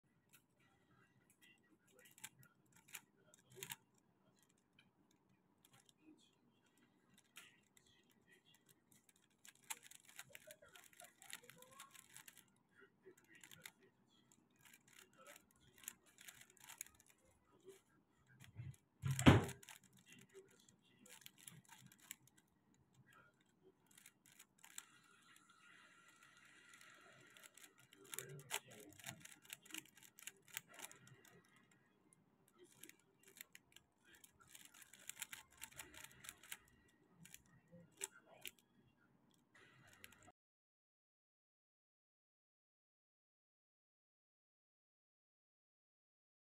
7.84 official PR single reconstruction!
Scramble F2 D' R' F' D2 R' B2 D2 F2 R2 D2 B L' U2 R' D L' D
Solution :
F U' R2 - Cross-1
U' L' U L U L' U L U' L' U L - 1st pair
y R U R' r U2 r' - Last cross piece + 2nd formed pair
y' R' U R - Insert 2nd pair
U' L U L' - 3rd pair
U U R U' R' U' R U' R' U R U' R' - 4th pair
U U r' R2 U R' U R U2 R' U M' - OLL
U U' U' - AUF